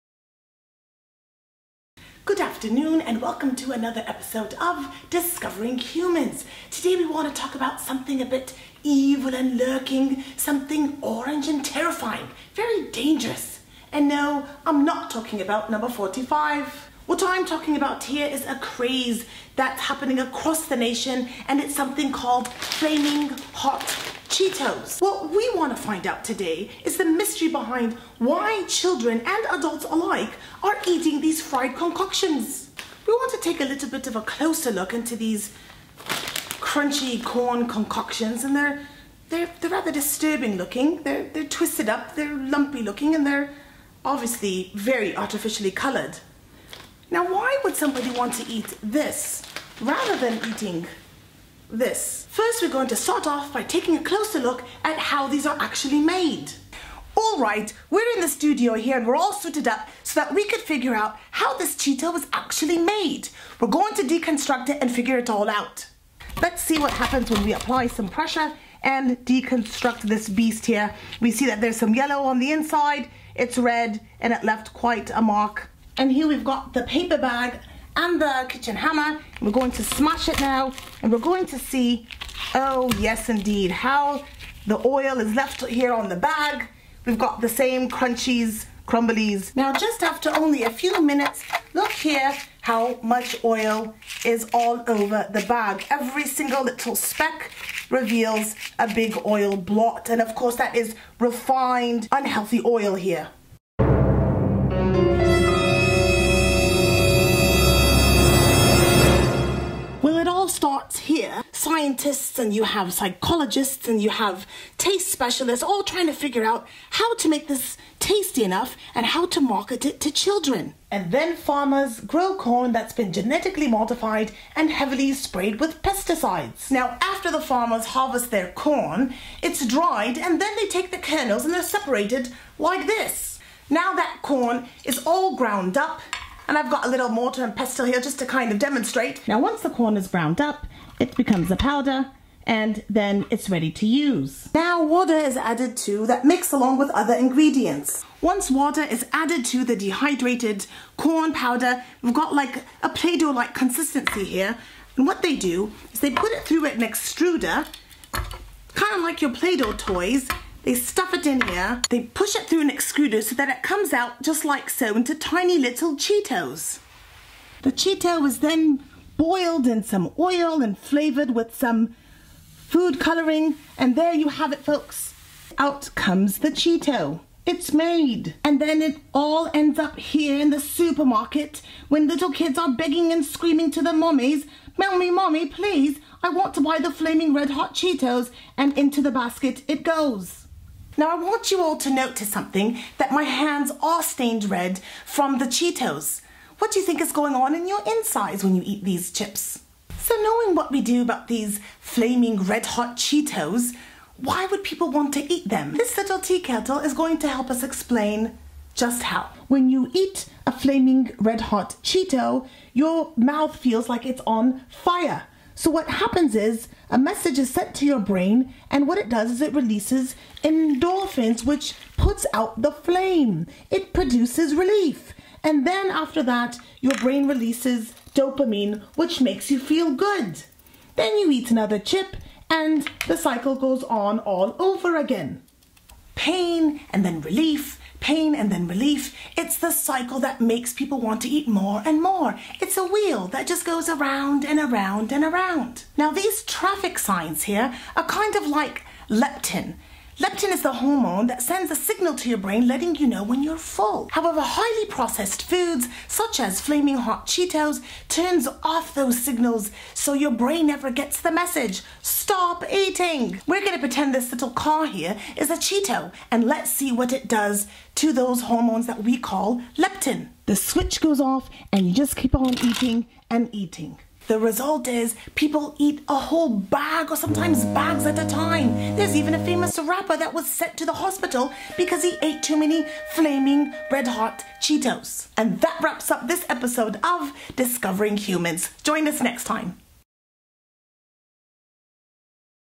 What's in Those Cheetos?
This video is about what Flaming Hot Cheetos are made of and how they affect your body.